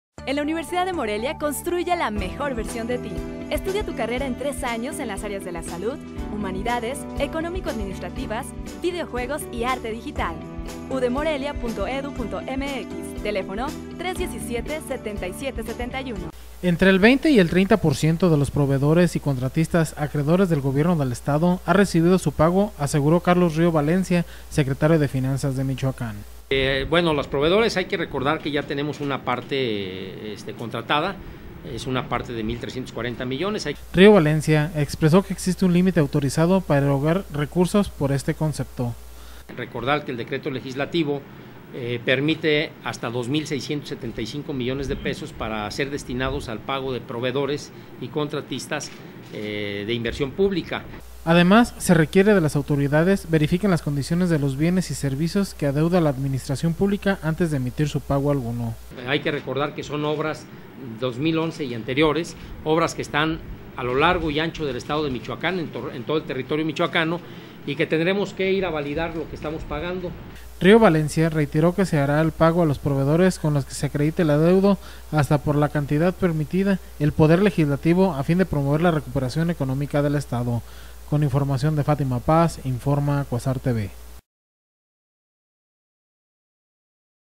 Reanudan gobierno pagos a contratistas y proveedores: Río
Entre el 20 y el 30 por ciento de los proveedores y contratistas acreedores del Gobierno del Estado ha recibido su pago, aseguró Carlos Río Valencia, secretario de Finanzas de Michoacán...